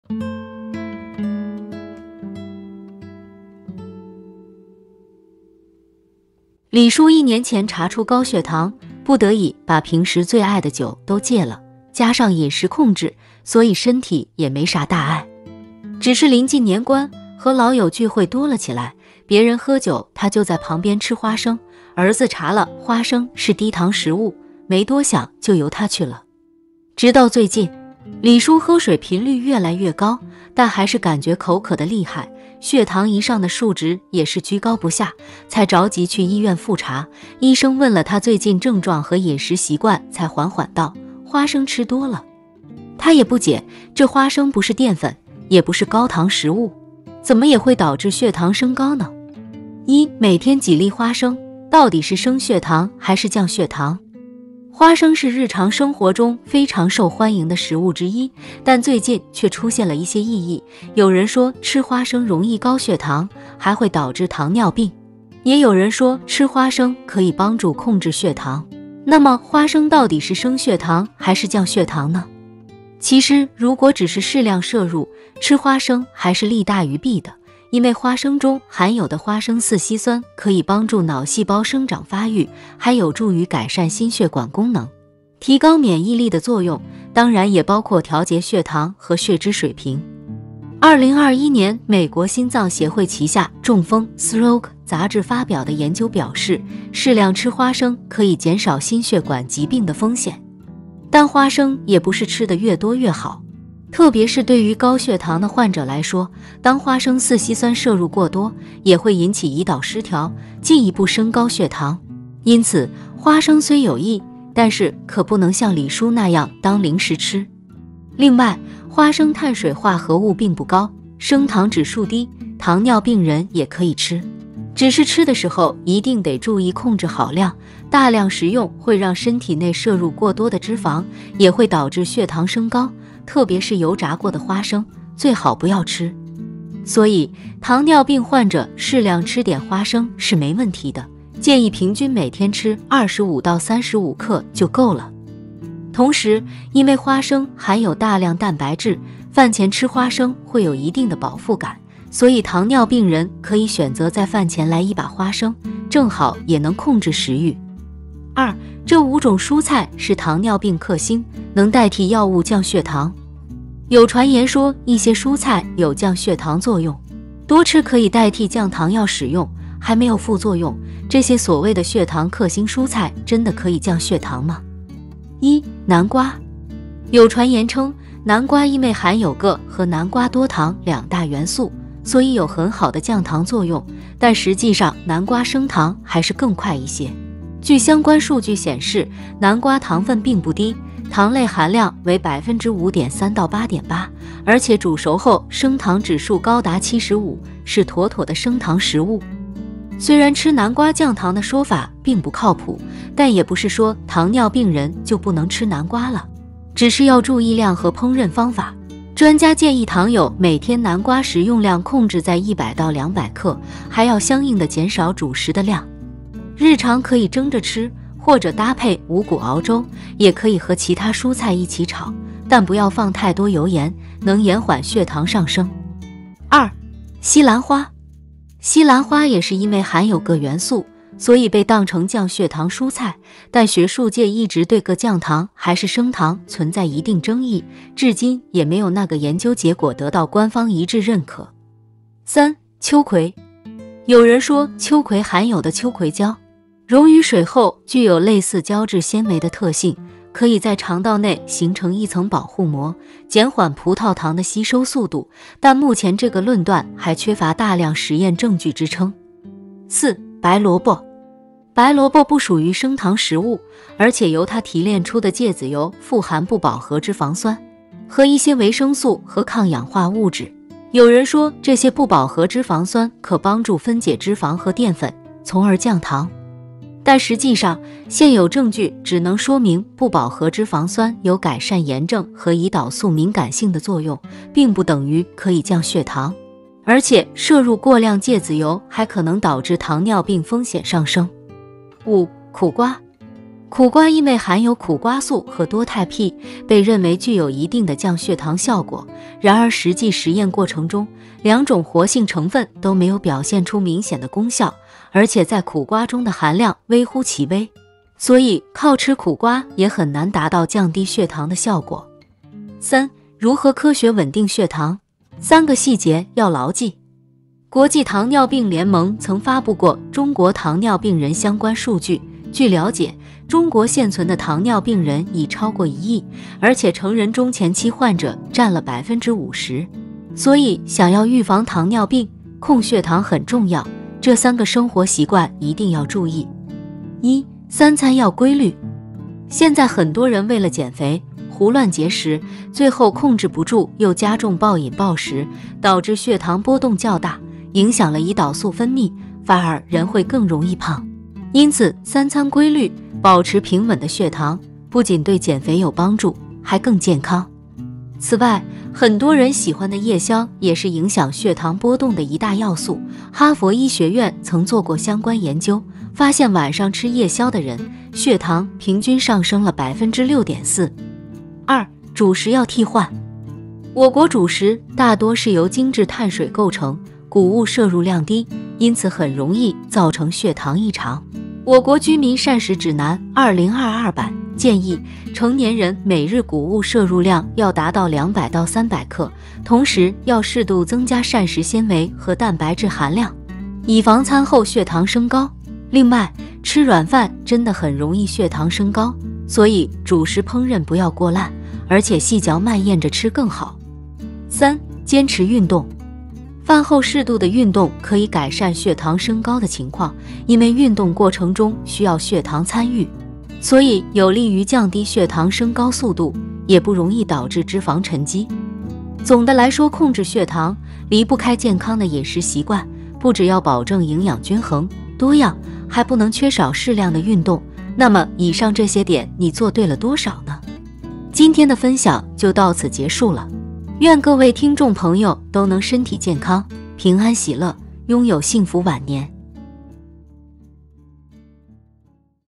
糖尿病最怕這5種蔬菜，不吃藥也能降血糖？ 別再讓父母受騙了。【幸福Talks】中老年心語
唯美頻道，健康科普，关注中老年健康，願天下中老年人晚年幸福安康~

糖尿病最怕這5種蔬菜，不吃藥也能降血糖？ 別再讓父母受騙了。